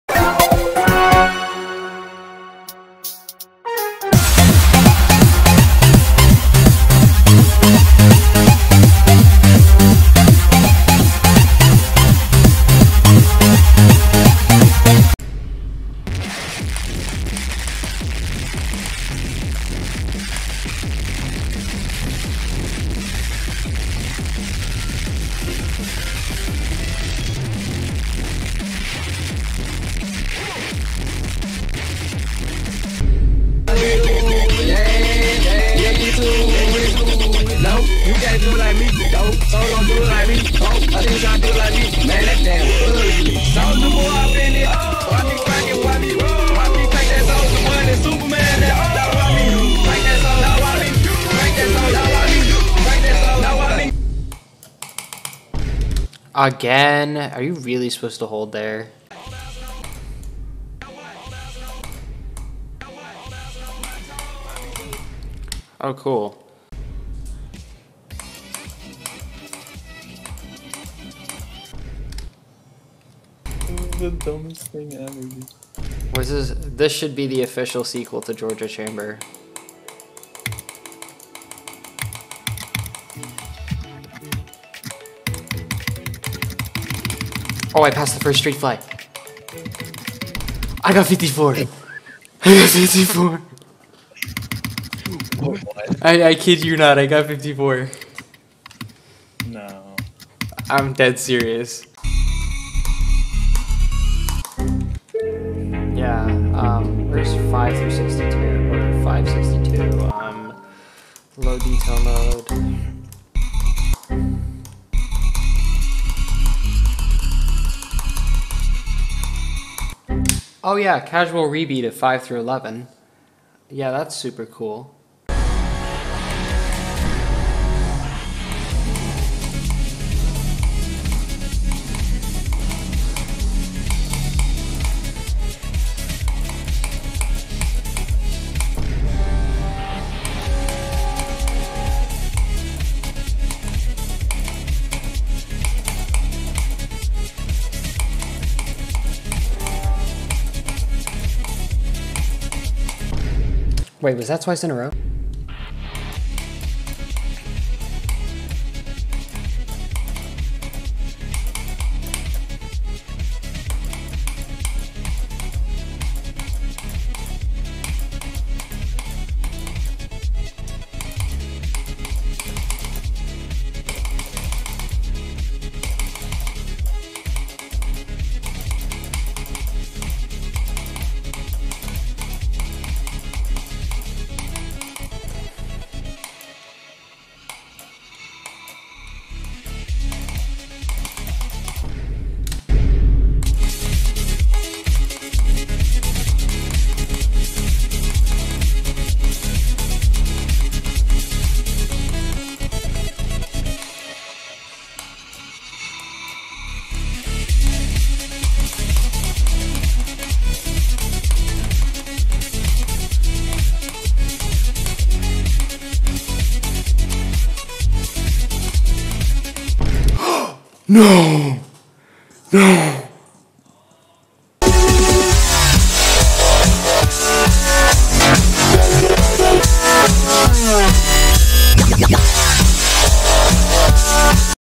Ultimate Filler Run Compilation
Busy still. As I promised. Here are timestamps and thoughts on what I did:

0:15 Binbo crea el daily: Binbo Drink lava
0:32 SC66p 17-100: Free. Not to be confused with SC66pp
0:53 Denouement update 1st wave: Did this 3x. Dumb transition why'd you change it Synactive?
1:00 DIAMOND DANCE 40%: passed hard part
1:07 FINAL DESTINATION 38%: I've done better but this is my best run on the official
1:16 BTS ARMY quiz 54%: WAY harder than it looks. By the real FUWM
1:41 Silent Clubstep 1st ship 5th time: ok then
2:06 Silent Clubstep 24-33+ 2x in a row: Background noise = succ concentration. Surprised I did that. Almost did it 2 more times.
2:34 Silent Clubstep 24-66: A mile harder than 24-63/4
3:12 Silent Clubstep 48-78: Did this twice.